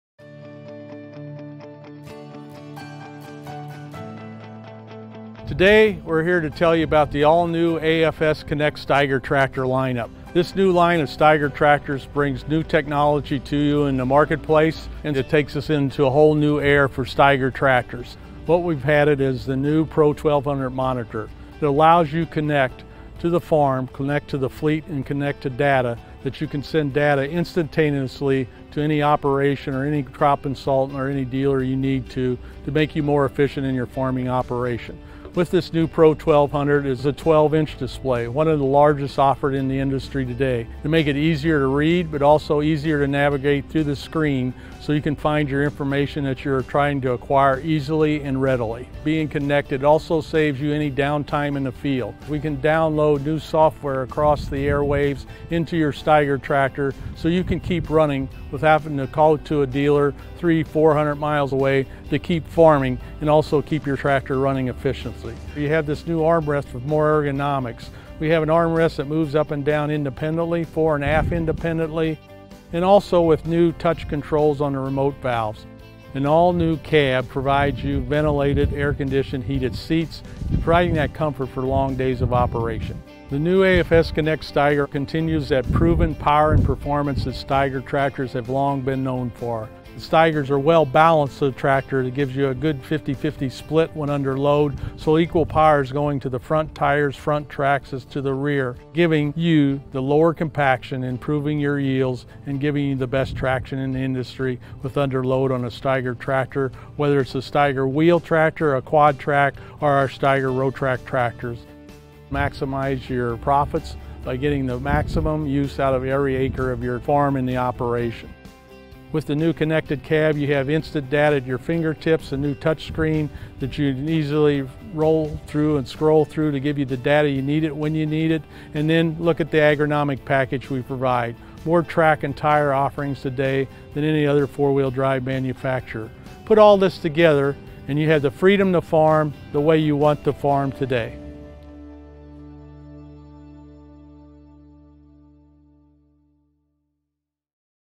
AFS Connect Steiger Series Tractors with Mitch Kaiser
The AFS Connect Steiger brings you the latest technology, powered by AFS Connect, allowing you to seamlessly connect to the farm, your fleet and your data. Featuring a completely redesigned cab with enhanced ergonomic comfort, the all new AFS Vision Pro operating system and new 12” AFS Pro 1200 display.  The AFS Connect Steiger gives you the freedom to farm the way you want to farm.